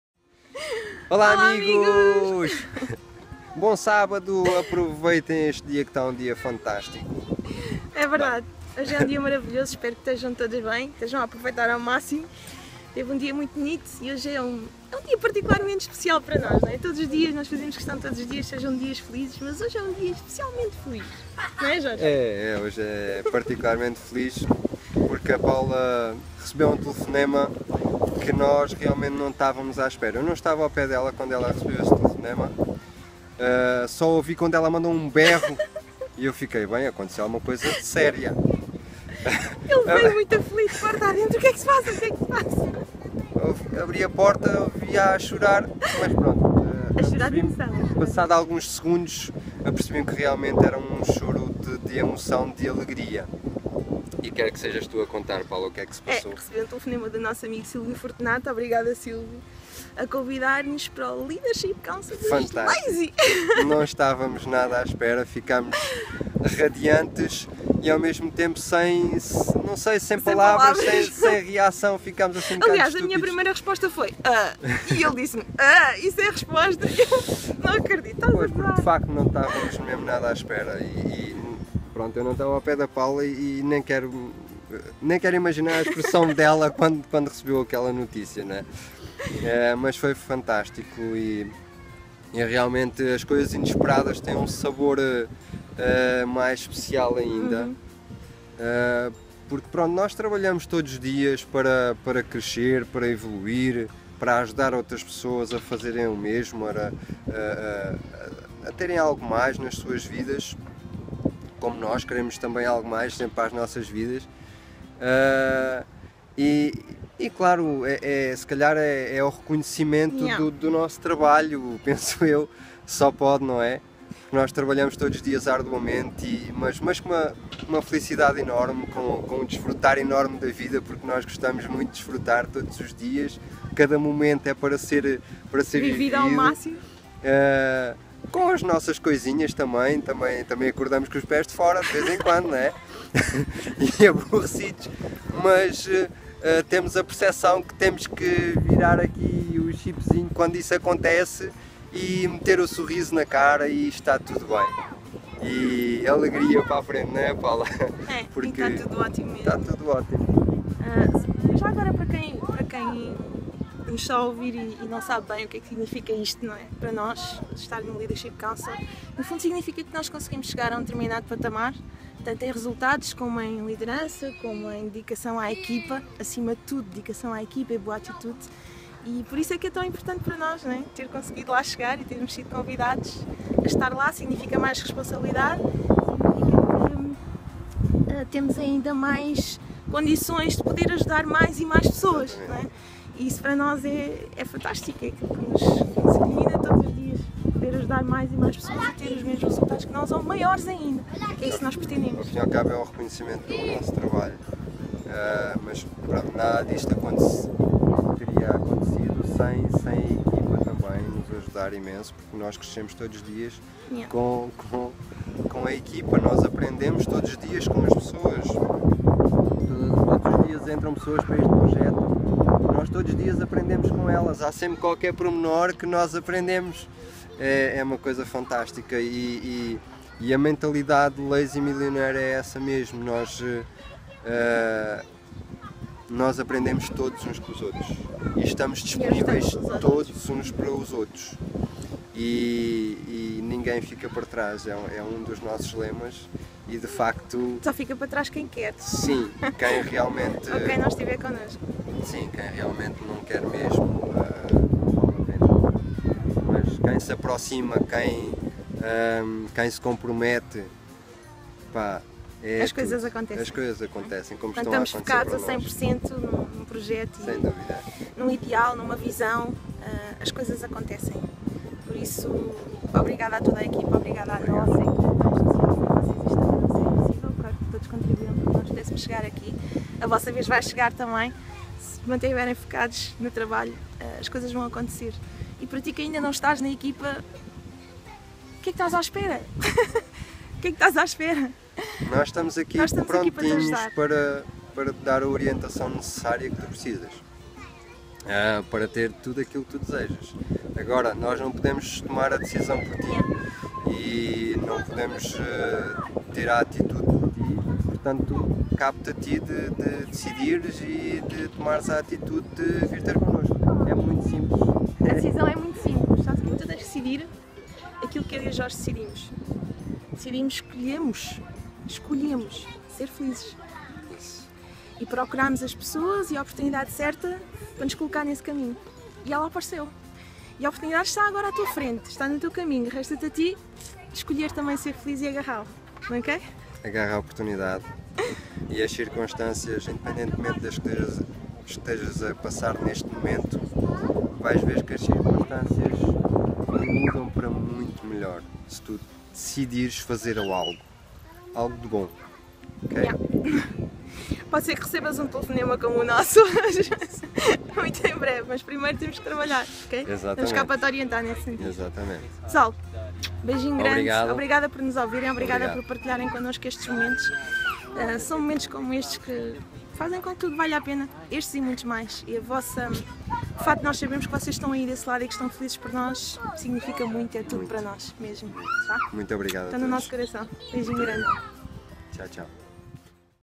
Estamos no Leadership Council! Yessssssssss
Estar no Leadership Council significa o reconhecimento pelo nosso trabalho e dedicação a este projeto:

E pensar que tudo começou com a decisão de REAMENTE mudar a nossa vida e lutar pelos nossos SONHOS em vez de trabalhar para o sonhos dos outros.

Se tens sonhos por realizar, vontade de aprender e humildade em seguir instruções, junta-te à nossa equipa:

Vamos te dar todo o apoio e formação que precisas para aprenderes como trabalhar na internet de forma profissional e atingir os teus objetivos.